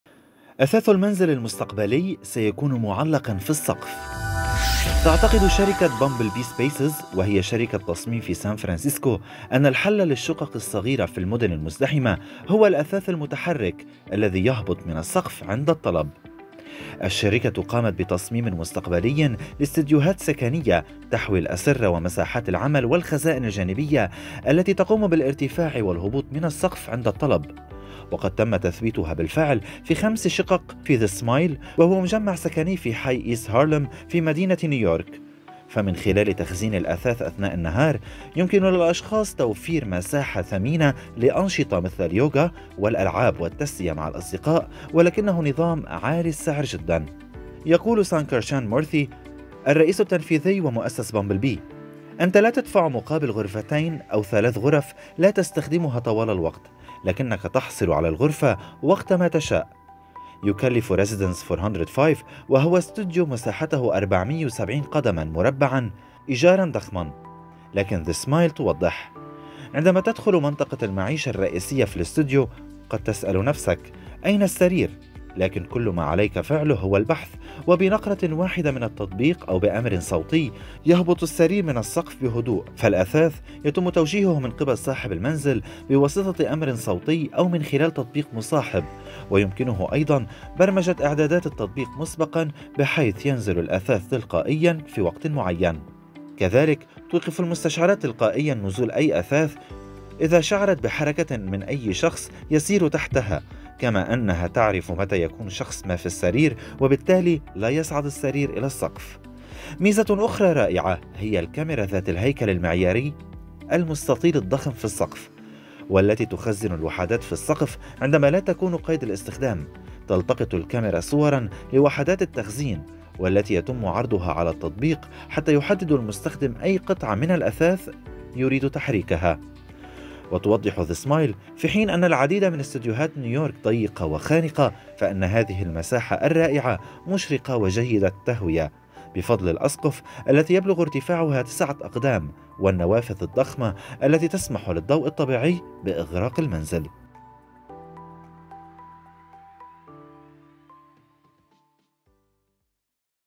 أثاث المنزل المستقبلي سيكون معلّقاً في السقف
تعتقد شركة Bumblebee Spaces وهي شركة تصميم في سان فرانسيسكو أن الحل للشقق الصغيرة في المدن المزدحمة هو الأثاث المتحرك الذي يهبط من السقف عند الطلب!!
الشركة قامت بتصميم مستقبلي لاستديوهات سكنيّة تحوي الأسرّة ومساحات العمل والخزائن الجانبية التي تقوم بالارتفاع والهبوط من السقف عند الطلب ، وقد تم تثبيتها بالفعل في خمس شقق في The Smile ، وهو مجمع سكني في حي East Harlem في مدينة نيويورك. فمن خلال تخزين الأثاث أثناء النهار ، يمكن للأشخاص توفير مساحة ثمينة لأنشطة مثل اليوجا والألعاب والتسلية مع الأصدقاء, ولكنه نظام عالِ السعر جداً
يقول Sankarshan Murthy ، الرئيس التنفيذي ومؤسس Bumblebee: "أنت لا تدفع مقابل غرفتين أو ثلاث غرف لا تستخدمها طوال الوقت ، لكنك تحصل على الغرفة وقتما تشاء".
يكلف Residence 405 وهو استوديو مساحته 470 قدماً مربعة إيجارًا ضخمًا لكن The Smile توضّح: "عندما تدخل منطقة المعيشة الرئيسية في الاستوديو ، قد تسأل نفسك ،" أين السرير؟  لكن كل ما عليك فعله هو البحث! وبنقرة واحدة من التطبيق أو بأمر صوتي يهبط السرير من السقف بهدوء
فالأثاث يتم توجيهه من قبل صاحب المنزل بواسطة أمر صوتي أو من خلال تطبيق مصاحب, ويمكنه أيضاً برمجة إعدادات التطبيق مسبقًا بحيث ينزل الأثاث تلقائيًا في وقت معين.
كذلك توقف المستشعرات تلقائيًا نزول أي أثاث إذا شعرت بحركة من أي شخص يسير تحتها كما أنها تعرف متى يكون شخص ما في السرير - وبالتالي لا يصعد السرير إلى السقف
ميزة أخرى رائعة هي الكاميرا ذات الهيكل المعياري المستطيل الضخم في السقف ، والتي تخزن الوحدات في السقف عندما لا تكون قيد الاستخدام. تلتقط الكاميرا صورًا لوحدات التخزين والتي يتم عرضها على التطبيق ، حتى يحدد المستخدم أي قطعة من الأثاث يريد تحريكها
وتوضّح The Smile :"في حين أن العديد من استوديوهات نيويورك ضيقة وخانقة ، فإن هذه المساحة الرائعة مشرقة وجيدة التهوية ، بفضل الأسقف التي يبلغ ارتفاعها تسعة أقدام والنوافذ الضخمة التي تسمح للضوء الطبيعي بإغراق المنزل."

المصدر: Daily Mail